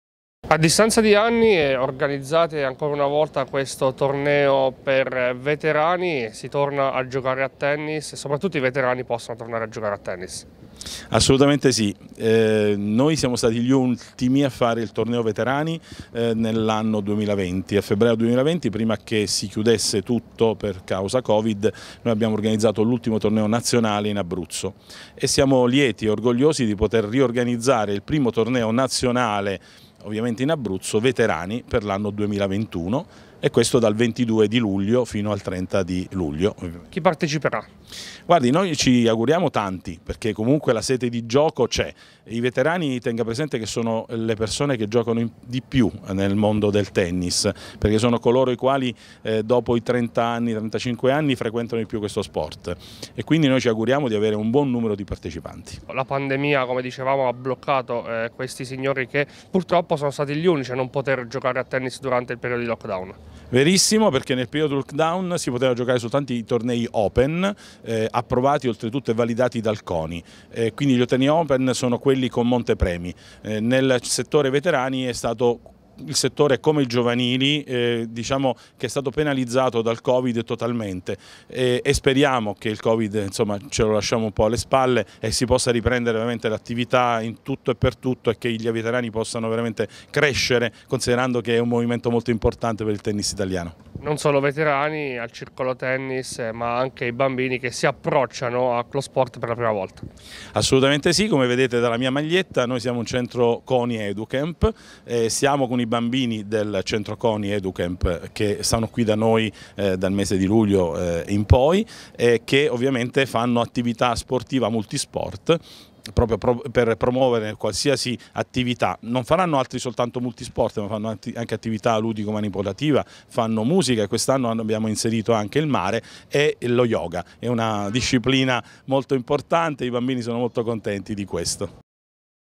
Tennis. Dal 22 luglio parte il torneo “Veterani”
Torna sui campi del Centro Polisportivo di Marina di Città Sant’Angelo il torneo nazionale di Tennis per veterani organizzato anche quest’anno dal Circolo Tennis Silvi. Il via alle gare è fissato per il 22 luglio